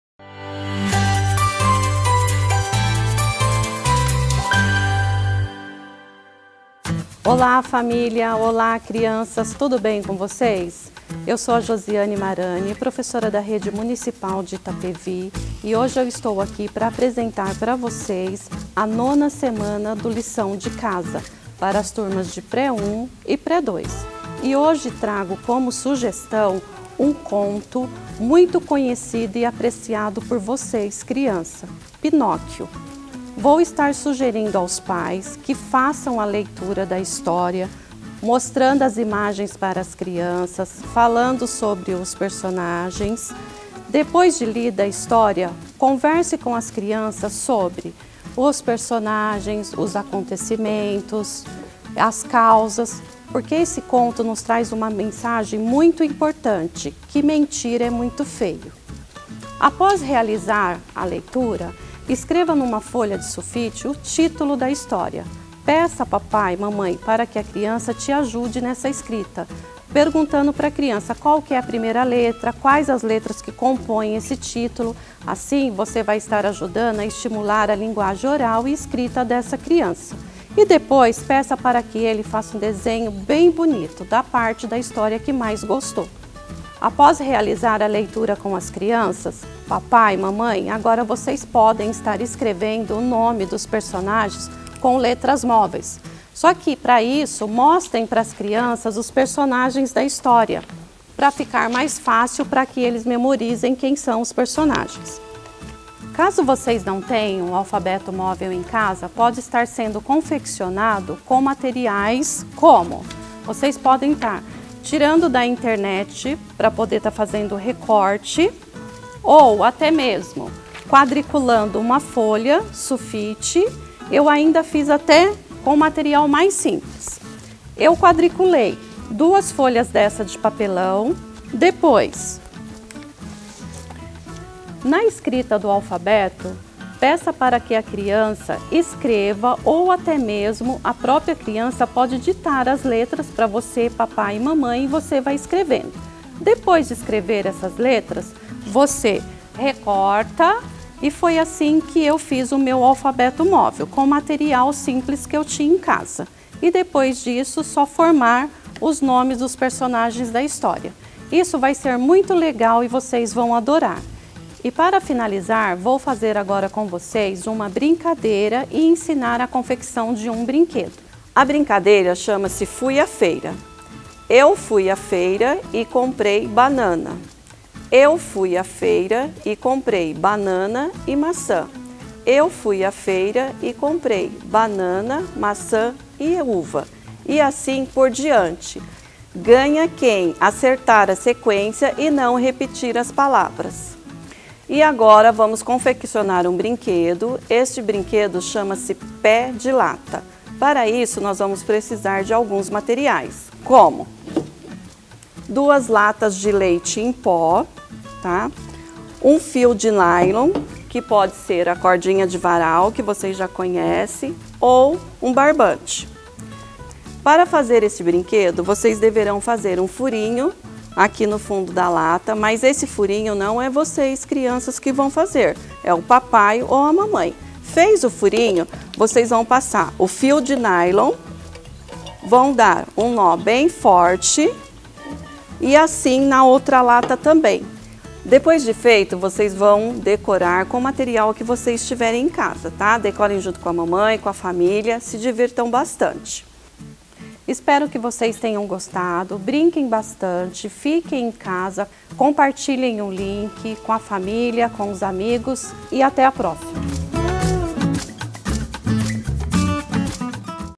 LIÇÃO DE CASA - VIDEOAULA 09 PARA OS ALUNOS DO PRÉ 1 E PRÉ 2
Acompanhe a 9ª videoaula para os alunos do Pré 1 e Pre 2, com a professora Josiane Marani.
Boa aula!